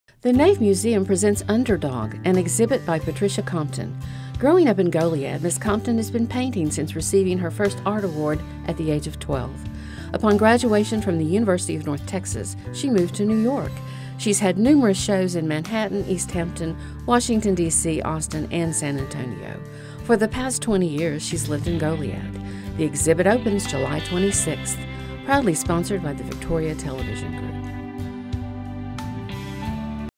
u n d e r d o g By: Patricia Compton
The Nave Museum proudly presents u n d e r d o g By: Patricia Compton.

     Born in Refugio, Texas, raised in Goliad, Texas:Patricia Compton has been painting since receiving her first art award from the Texas Department of Culture, Austin, at the age of 12. Compton graduated from the University of North Texas with a BFA in Design. Upon graduation she won an international design

competition which awarded her the funding to move to NYC.

     Her art career began with numerous group shows in Manhattan and Washington D.C., including The Pace Collection, Washington. She also has had solo shows in East Hampton, NY as well as various galleries in Austin, San Antonio, and Alamo Heights, Texas. The Nave Museum will be her first Museum show.

     Upon publication in David Carson’s “Speak” she decided to make the move out of NYC and “hole up” somewhere to paint a large new body of work. Goliad seemed like a good choice so Patricia came back home. Her painting career literally went to the dogs upon picking up her first stray, mangy dog off the steps of the Presidio La Bahia in Goliad and turned into 20 years of rescue, rehabilitation, and the adopting out of hundreds upon hundred of puppies and dogs to loving and deserving homes.

     She believes her gift for rescue is as much a God given gift as the ability to paint. She is now beginning a new body of commissioned work and determined, with the help of her visionary friends and supporters in rescue to get something built in Goliad reflective of art, dogs, rescue, and education on both subjects.